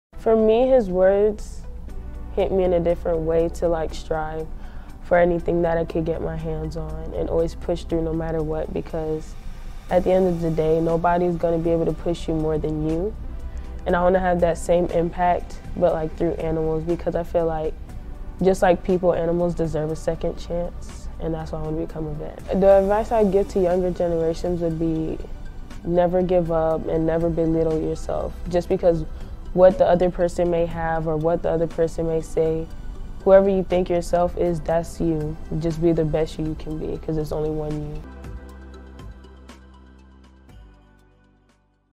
Black History Month: "Just be the best you, you can be"
Khaliah Moore is a senior at Evans High School in Orlando and one of this year's Arthur "Pappy" Kennedy Scholarship recipients. Her goal is to become a veterinarian.

Khaliah recently told us what Martin Luther King, Jr.'s legacy means to her and future generations.